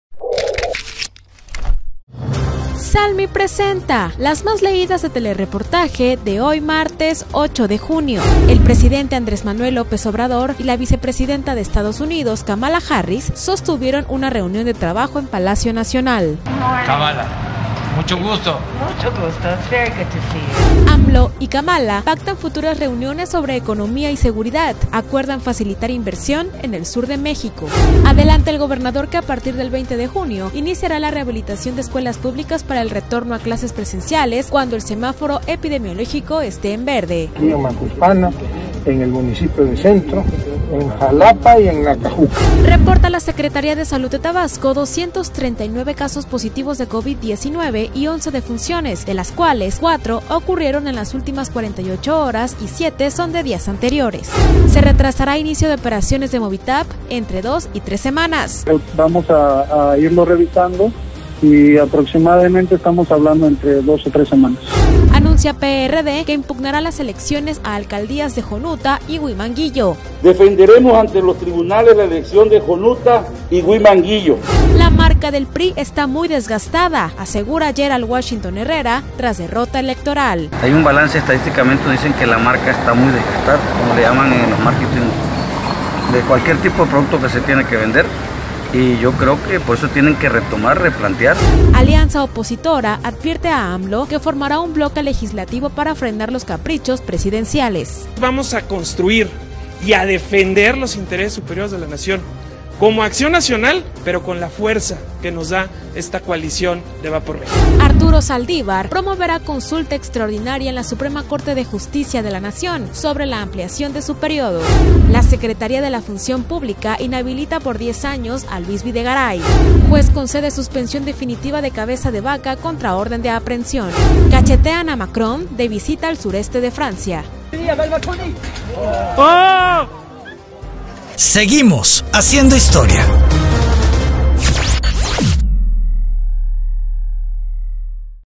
LasMásLeídas de Telereportaje del 08 de junio
🔴AMLO y Kamala Harris se reúnen en Palacio Nacional

🔴 Economía y seguridad, temas para las siguientes reuniones bilaterales

🔴 En junio rehabilitarán escuelas en Tabasco

🔴 Alianza opositora dice que frenará "caprichos presidenciales"